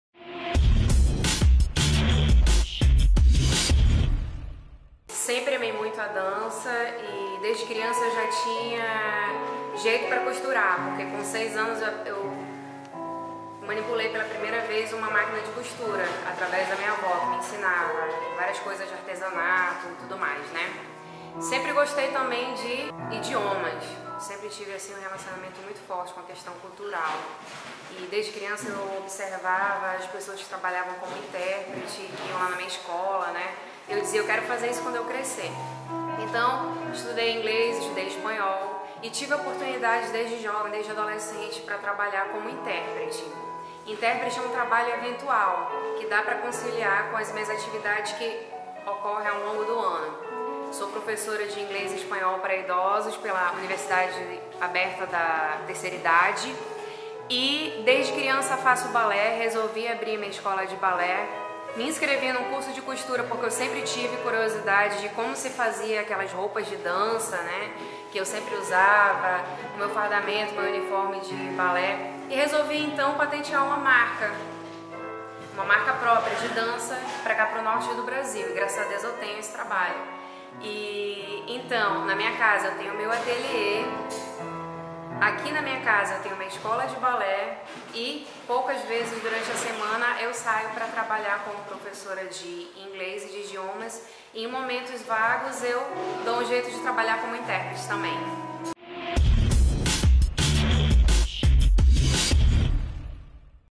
Profissional polivalente
A dona da Escola de Ballet Clássico Cia de Dança Encontro das Águas, Juliana Borges conta como seguiu diferentes ramos e os conciliou.